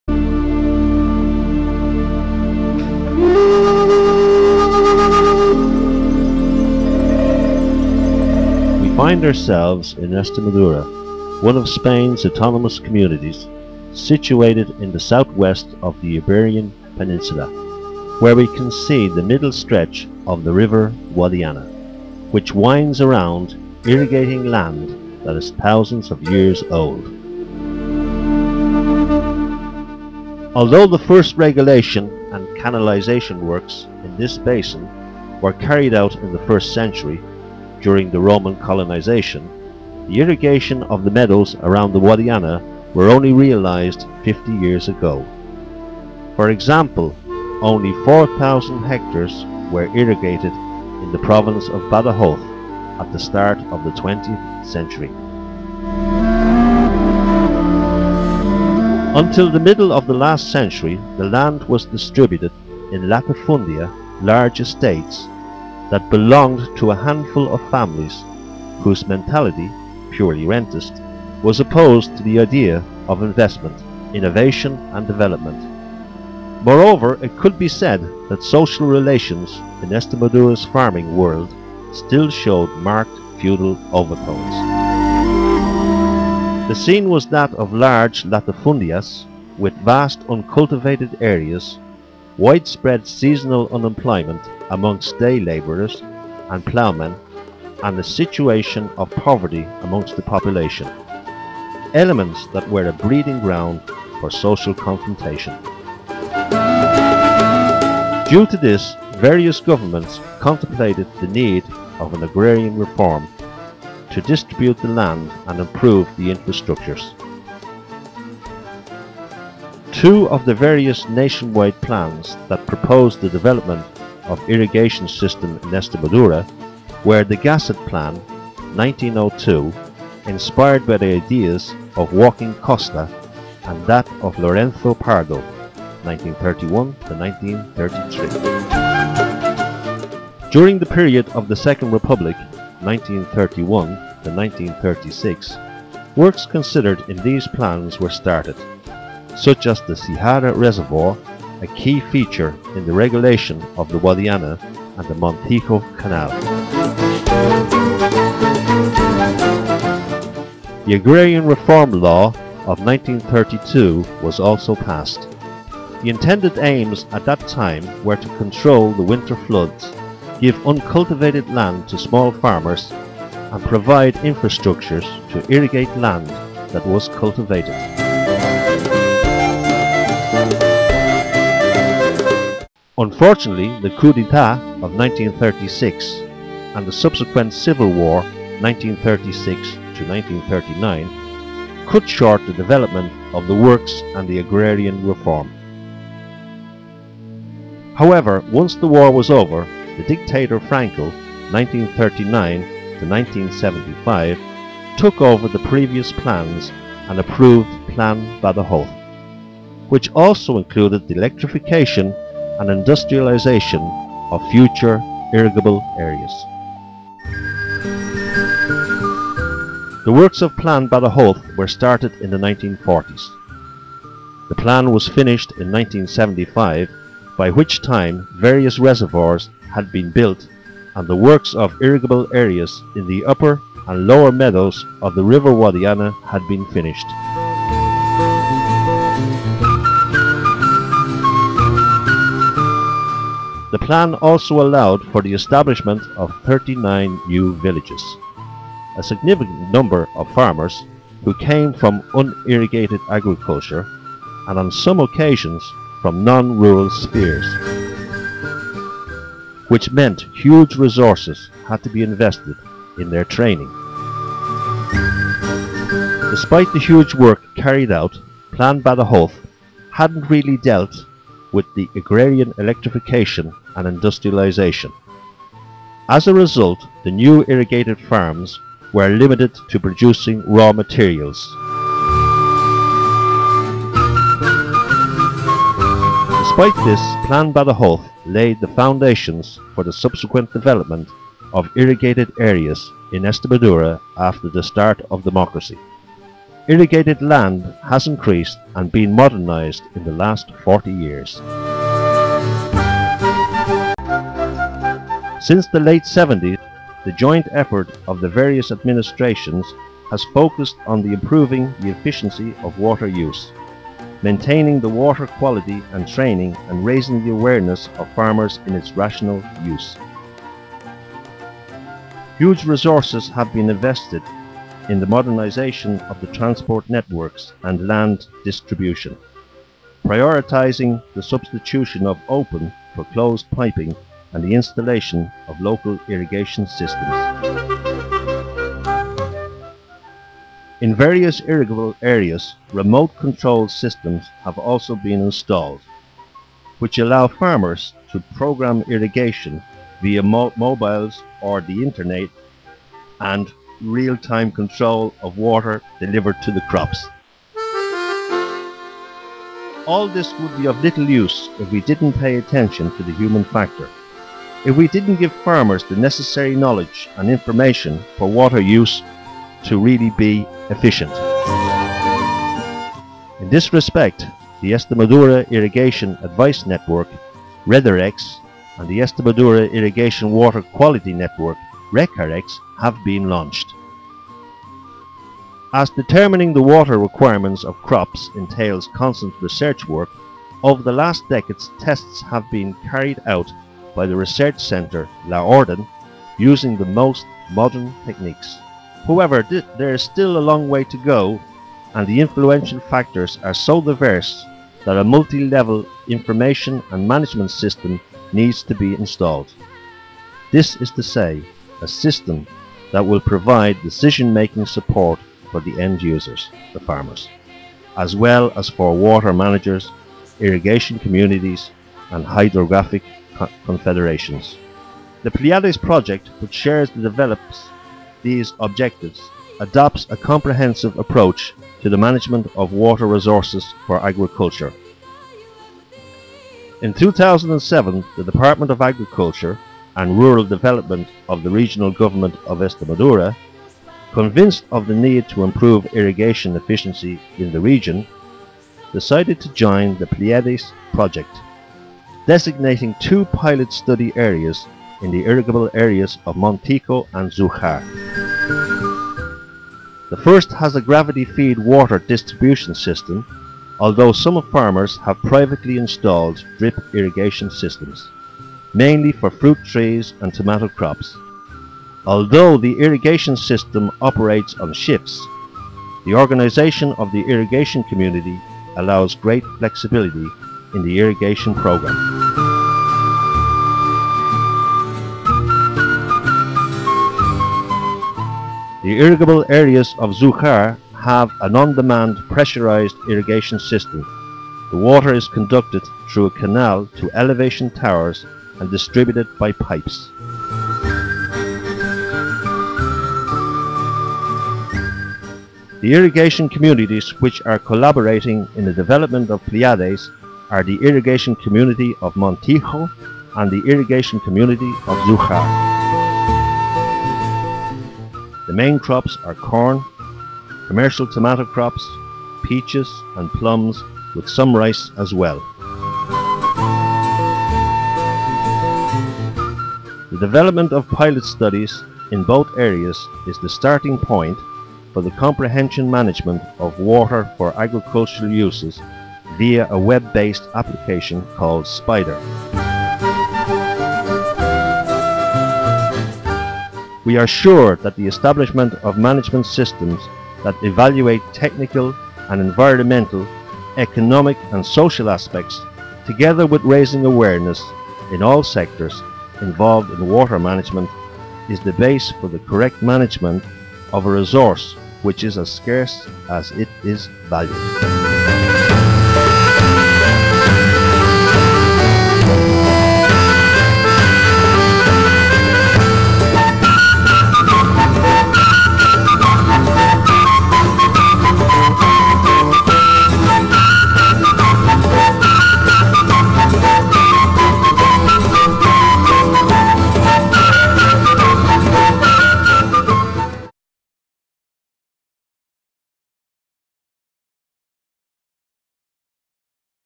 Las Vegas del Guadiana Pilot Story (PLEIADeS Spain)
Las Vegas del Guadiana, located in the Guadiana river basin (Spain), is one of the nine pilot areas of the project PLEIADeS This video tells the story of the past and current situation of water resources and irrigation in the area.